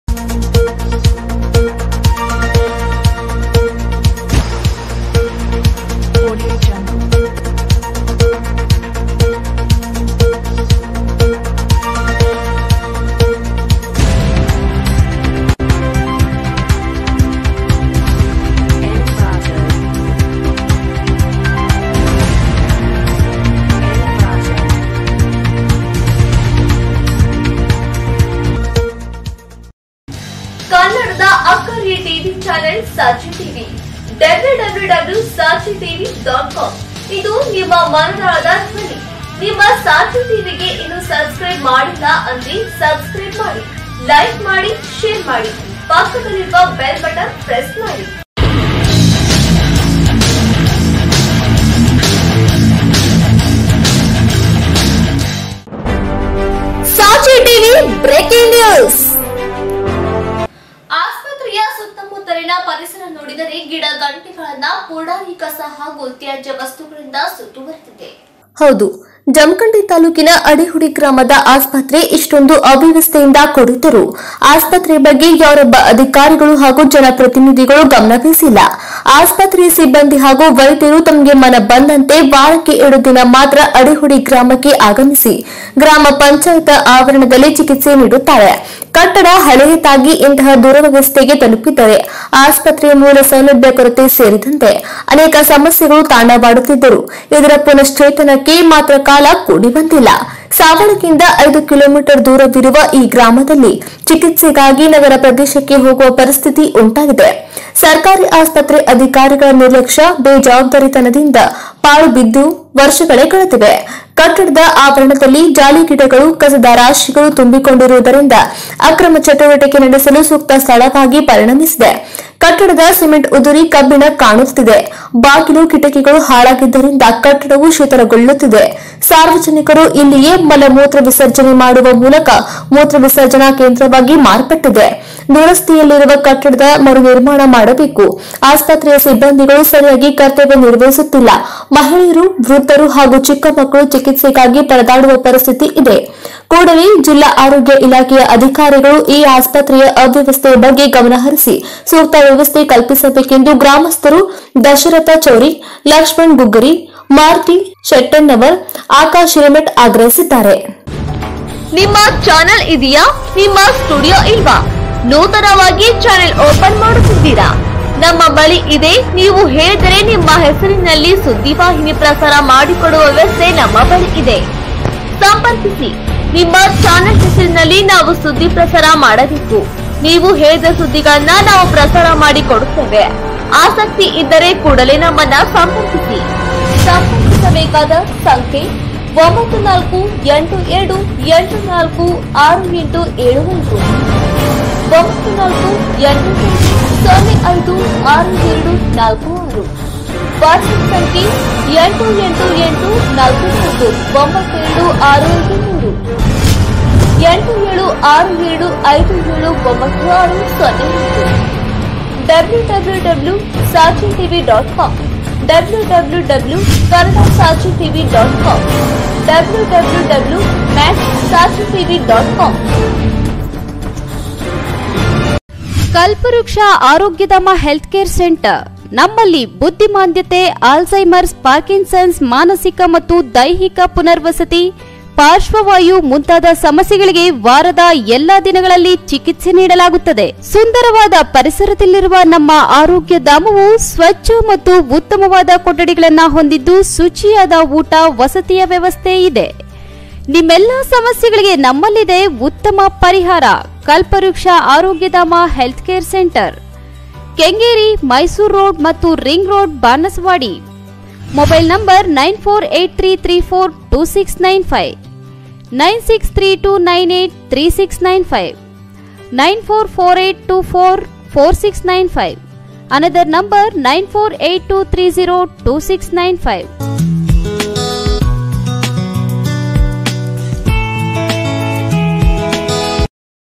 ಆಸ್ಪತ್ರೆಯ ಸುತ್ತಮುತ್ತಲಿನ ಪರಿಸರ |ಕಸ ಹಾಗು ತ್ಯಾಜ್ಯ ವಸ್ತುಗಳಿಂದ| sachi tv
ಆಸ್ಪತ್ರೆಯ ದುಸ್ಥಿತಿ : ರೋಗಿಗಳಿಗೆ ಪರದಾಟ
ಶೀಥಿಲಾವಸ್ಥೆಯಲ್ಲಿರುವ ಅಡಿಹುಡಿ ಸರ್ಕಾರಿ ಆಸ್ಪತ್ರೆಯ ಕಟ್ಟಡ

ಸಾವಳಗಿ : ಅದೊಂದು ಆಸ್ಪತ್ರೆಯ ಕಟ್ಟಡ. ಅನಾರೋಗ್ಯವೆಂದು ಇಲ್ಲಿಗೆ ಬಂದವರು ನಿಜಕ್ಕೂ ಒಂದು ಕ್ಷಣ ಗಾಬರಿಯಾಗಲೇಬೇಕು. ಏಕೆಂದರೆ ಈ ಆಸ್ಪತ್ರೆಯ ಸುತ್ತಮುತ್ತಲಿನ ಪರಿಸರ ನೋಡಿದರೆ ಗಿಡಗಂಟಿಗಳು, ಪುಡಾರಿ ಕಸ ಹಾಗು ತ್ಯಾಜ್ಯ ವಸ್ತುಗಳಿಂದ ಸುತ್ತುವರೆದಿದೆ. ಆಸ್ಪತ್ರೆಯ ಕಟ್ಟಡ ಎಲ್ಲಿ ಕುಸಿದು ಬೀಳುತ್ತದೋ ಎಂಬ ಆತಂಕ ಮೂಡುತ್ತದೆ.
ಹೌದು ಜಮಖಂಡಿ ತಾಲ್ಲೂಕಿನ ಅಡಿಹುಡಿ ಗ್ರಾಮದ ಆಸ್ಪತ್ರೆ  ಇಷ್ಟೊಂದು ಅವ್ಯವಸ್ಥೆಯಿಂದ ಕೂಡಿದ್ದರೂ ಆಸ್ಪತ್ರೆಯ ಬಗ್ಗೆ ಯಾರೊಬ್ಬ ಅಧಿಕಾರಿಗಳು ಹಾಗು ಜನಪ್ರತಿನಿಧಿಗಳು ಗಮನವಹಿಸಿಲ್ಲ. ಆಸ್ಪತ್ರೆಯ ಸಿಬ್ಬಂದಿ ಹಾಗು ವೈದ್ಯರು ತಮಗೆ ಮನ ಬಂದಂತೆ ವಾರಕ್ಕೆ ಎರಡು ದಿನ ಮಾತ್ರ ಅಡಿಹುಡಿ ಗ್ರಾಮಕ್ಕೆ ಆಗಮಿಸಿ ಗ್ರಾಮ ಪಂಚಾಯತ ಆವರಣದಲ್ಲಿ ಚಿಕಿತ್ಸೆ ನೀಡುತ್ತಾರೆ. ಕಟ್ಟಡ ಹಳೆಯದಾಗಿ ಇಂತಹ ದುರವಸ್ಥೆಗೆ ತಲುಪಿದ್ದರೆ. ಆಸ್ಪತ್ರೆಯ ಮೂಲ ಸೌಲಭ್ಯ ಕೊರತೆ ಸೇರಿದಂತೆ ಅನೇಕ ಸಮಸ್ಯೆಗಳು ತಾಂಡವಾಡುತ್ತಿದ್ದರೂ ಇದರ ಪುನಶ್ಚೇತನಕ್ಕೆ ಮಾತ್ರ ಕಾಲ ಕೂಡಿ ಬಂದಿಲ್ಲ.
ಸಾವಳಗಿಯಿಂದ 5 ಕಿ.ಮೀ. ದೂರವಿರುವ ಈ ಗ್ರಾಮದಲ್ಲಿ ಚಿಕಿತ್ಸೆಗಾಗಿ ನಗರ ಪ್ರದೇಶಕ್ಕೆ ಹೋಗುವ ಪರಿಸ್ಥಿತಿ ಉಂಟಾಗಿದೆ. ಸರ್ಕಾರಿ ಆಸ್ಪತ್ರೆ ಅಧಿಕಾರಿಗಳ ನಿರ್ಲಕ್ಷ, ಬೇಜವಾಬ್ದಾರಿಯಿಂದ ಪಾಳು ಬಿದ್ದು ವರ್ಷಗಳೇ ಕಳೆದಿವೆ. ಕಟ್ಟಡದ ಆವರಣದಲ್ಲಿ ಜಾಲಿಗಿಡಗಳು, ಕಸದ ರಾಶಿಗಳು ತುಂಬಿಕೊಂಡಿರುವದರಿಂದ ಅಕ್ರಮ ಚಟುವಟಿಕೆ ನಡೆಸಲು ಸೂಕ್ತ ಸ್ಥಳವಾಗಿ ಪರಿಣಮಿಸಿದೆ.
ಕಟ್ಟಡದ ಸಿಮೆಂಟ ಉದುರಿ ಕಬ್ಬಿಣ ಕಾಣುತ್ತಿವೆ. ಬಾಗಿಲು, ಕಿಟಕಿಗಳು ಹಾಳಾಗಿದ್ದರಿಂದ ಕಟ್ಟಡವು ಶೀಥಿಲಗೊಳ್ಳುತ್ತಿದೆ. ಸಾರ್ವಜನಿಕರು ಇಲ್ಲಿಯೇ ಮಲ ಮೂತ್ರ ವಿಸರ್ಜನೆ ಮಾಡುವ ಮೂಲಕ ಮೂತ್ರ ವಿಸರ್ಜನಾ ಕೇಂದ್ರವಾಗಿ ಮಾರ್ಪಟ್ಟಿದೆ.
ದುರಸ್ಥಿಯಲ್ಲಿರುವ ಕಟ್ಟಡದ ಮರುನಿರ್ಮಾಣ ಮಾಡಬೇಕು. ಆಸ್ಪತ್ರೆಯ ಸಿಬ್ಬಂದಿಗಳು ಸರಿಯಾಗಿ ಕರ್ತವ್ಯ ನಿರ್ವಹಿಸುತ್ತಿಲ್ಲ, ಮಹಿಳೆಯರು, ವೃದ್ದರು ಹಾಗು ಚಿಕ್ಕಮಕ್ಕಳು ಚಿಕಿತ್ಸೆಗಾಗಿ ಪರದಾಡುವ ಪರಿಸ್ಥಿತಿ ಇದೆ. ಕೂಡಲೆ ಜಿಲ್ಲಾ ಆರೋಗ್ಯ ಇಲಾಖೆಯ ಅಧಿಕಾರಿಗಳು ಈ ಆಸ್ಪತ್ರೆಯ ಅವ್ಯವಸ್ಥೆಯ ಬಗ್ಗೆ ಗಮನಹರಿಸಿ ಸೂಕ್ತ ವ್ಯವಸ್ಥೆ ಕಲ್ಪಿಸಬೇಕೆಂದು ಗ್ರಾಮಸ್ಥರಾದ ದಶರಥ ಚೌರಿ, ಲಕ್ಷ್ಮಣ ಗುಗ್ಗರಿ, ಮಾರುತಿ ಶೆಟ್ಟೆನ್ನವರ, ಆಕಾಶ ಹಿರೇಮಠ ಆಗ್ರಹಿಸಿದ್ದಾರೆ.
ಬಾಕ್ಸ : 1) : ಅಡಿಹುಡಿ ಉಪ ಆರೋಗ್ಯ ಕೇಂದ್ರದ ಕಟ್ಟಡ ಶೀಥಿಲಾವಸ್ಥೆಯಲ್ಲಿದ್ದು, ಹೊಸ ಕಟ್ಟಡಕ್ಕಾಗಿ ಜಿಲ್ಲಾ ಆರೋಗ್ಯ ಅಧಿಕಾರಿಗಳಿಗೆ ಪ್ರಸ್ತಾವನೆ ಸಲ್ಲಿಸಿದ್ದೆವೆ. ಅನುದಾನ ಬಂದ ನಂತರ ಹೊಸ ಕಟ್ಟಡ ಪ್ರಾರಂಭಿಸುತ್ತೆವೆ. : ಜಿ.ಎಸ್.ಗಲಗಲಿ : ತಾಲೂಕಾ ಆರೋಗ್ಯಾಧಿಕಾರಿಗಳು ಜಮಖಂಡಿ.
ಬಾಕ್ಸ : 2) : ಅಡಿಹುಡಿಯಲ್ಲಿನ ಆಸ್ಪತ್ರೆಯು ಅನೈತಿಕ ಚಟುವಟಿಕೆಗಳ ತಾಣವಾಗಿದೆ. ಸಿಬ್ಬಂದಿಗಳು ಸರಿಯಾಗಿ ಕರ್ತವ್ಯ ನಿರ್ವಹಿಸುವದಿಲ್ಲ. ಕಟ್ಟಡದ ತುಂಬೆಲ್ಲ ಕಸದ ರಾಶಿ ಬಿದ್ದಿದ್ದರೂ ಯಾರೊಬ್ಬರು ವಿಚಾರಿಸುತ್ತಿಲ್ಲ. ಶೀಘ್ರದಲ್ಲಿಯೇ ಸರ್ಕಾರಿ ಆಸ್ಪತ್ರೆಯನ್ನು ಪುನಶ್ಚೇತನಗೊಳಿಸಬೇಕು. : ಅಬುಬಕರ ಜಮಖಂಡಿ : ಕಾಂಗ್ರೇಸ ಮುಖಂಡರು